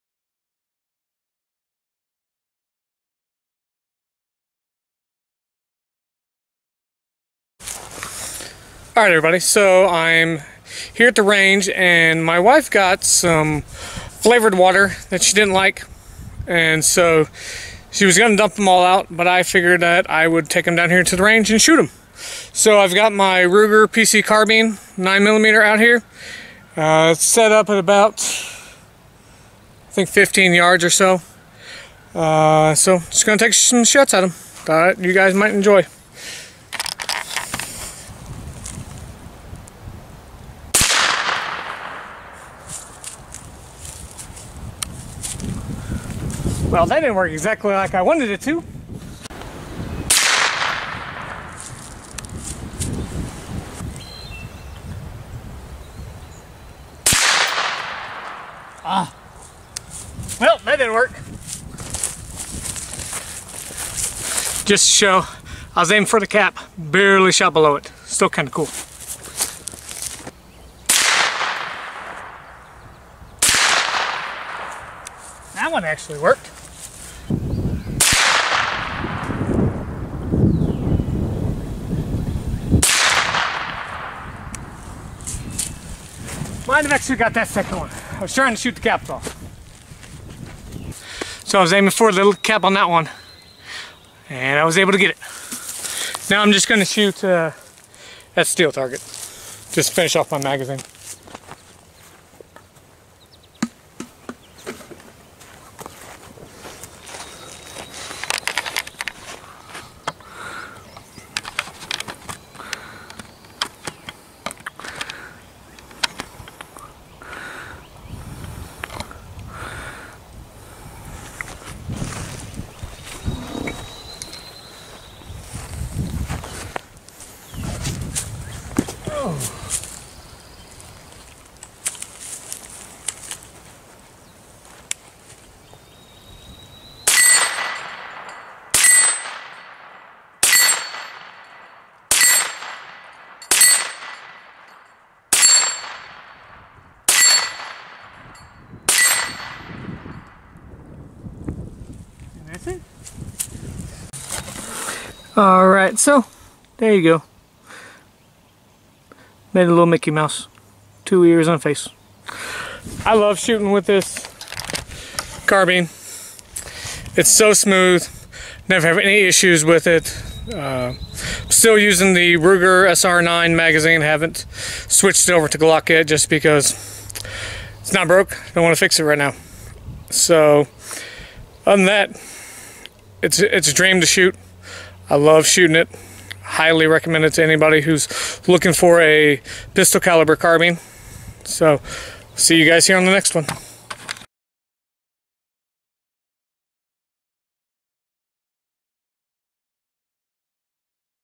Taking out some water bottles
Taking out some water bottles with my Ruger PC9 Carbine.  Lots of fun, highly recommend this to anyone looking for a pistol caliber carbine. Amazon Wish List Items